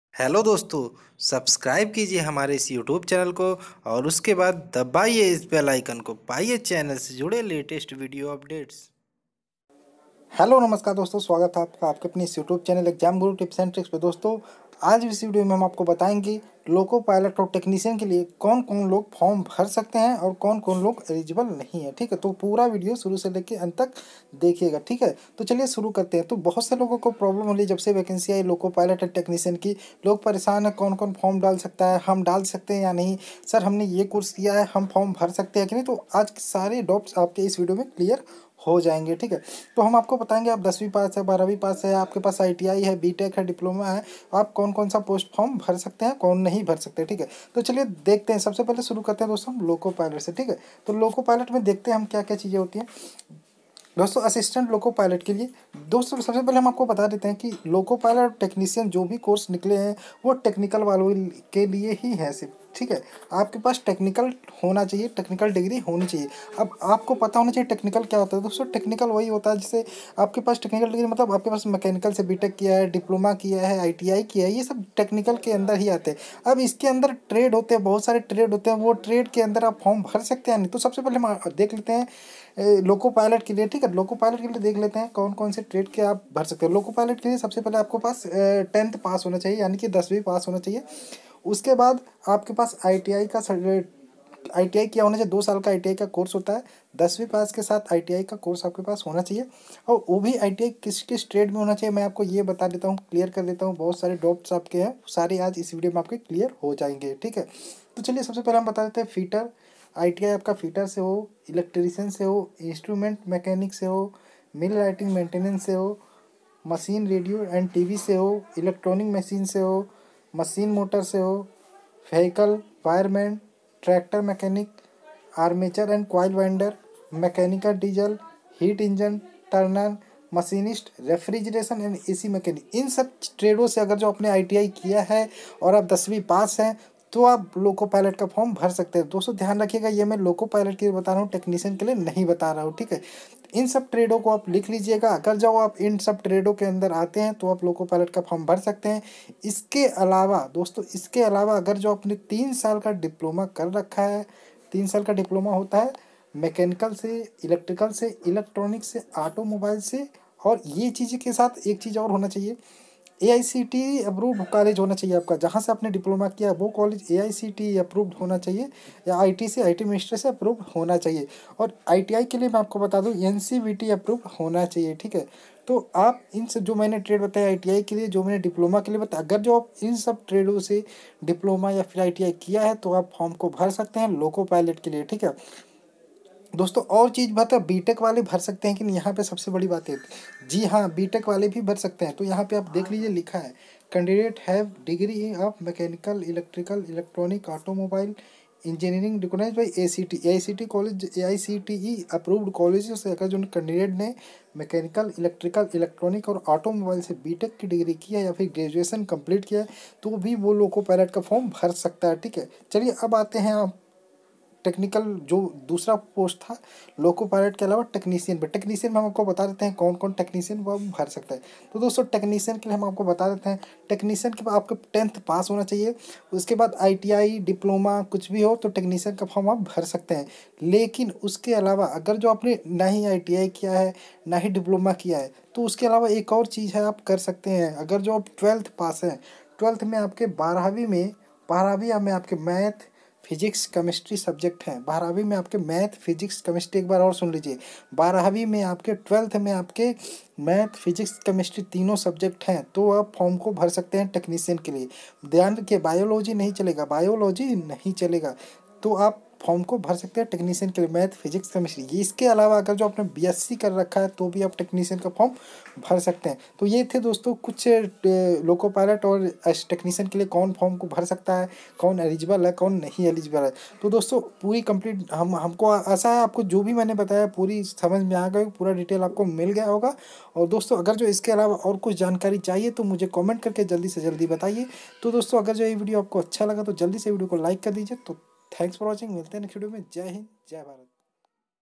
Railway Requirement Loco Pilot and Technician Elegebilty 10th,12th,iti,diploma,b tech
Railway Requirement Alp and Technician eligibility 10th,12th,iti, Diploma,b tech